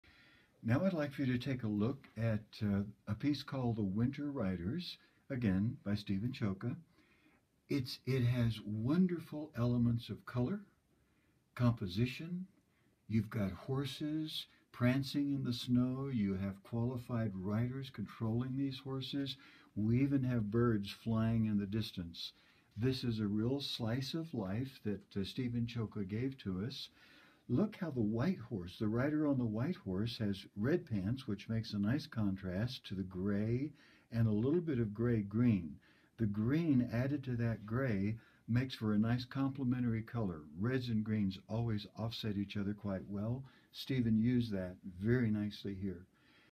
First Fridays Art Lecture, featuring Stephen Csoka_4
Highlights from First Fridays Art Lecture, February 6, 2015:  “Stephen Csoka—From Hungarian Treasures to Master Portraits and New York Landscapes”.